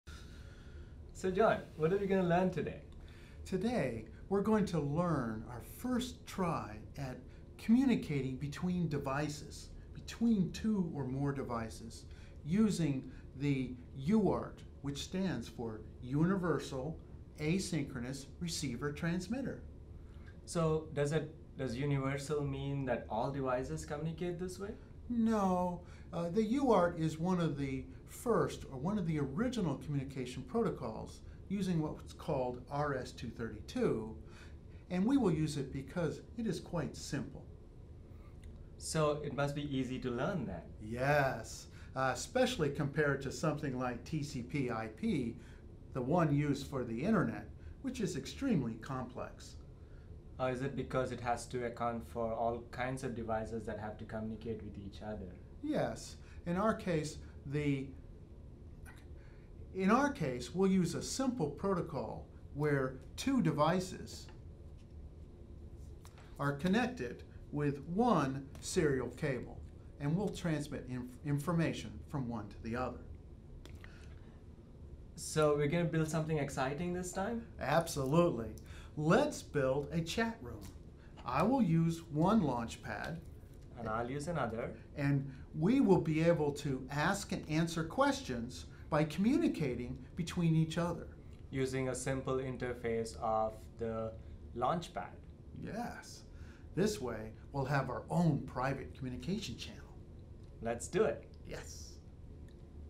C11 0 Introduction to serial interfacing
Professors Valvano and Yerraballi teach an online class on Embedded Systems. This video introduces Chapter 11.